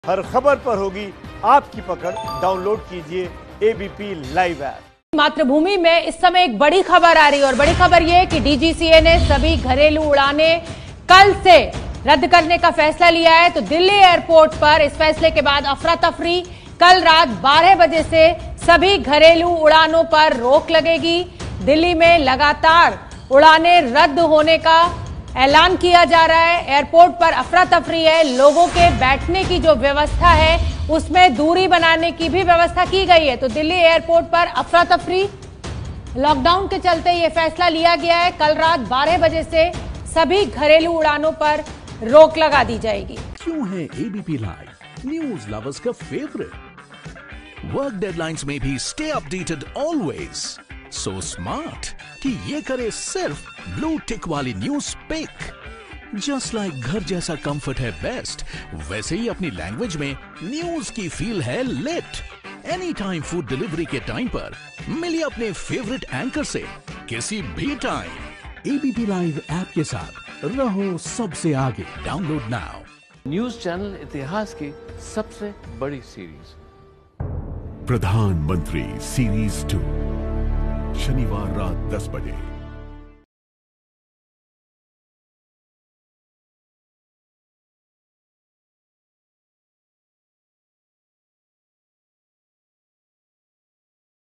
DGCA का फैसला- कल से सभी घरेलू उड़ानों पर रोक | ABP News Hindi
Corona के खतरे को देखते हुए DGCA ने कल से सभी घरेलू उड़ानों पर रोक लगाने का फैसला किया है. बता दें कि देश के लगभग 100 जिलों को लॉकडाउन में रखा गया है.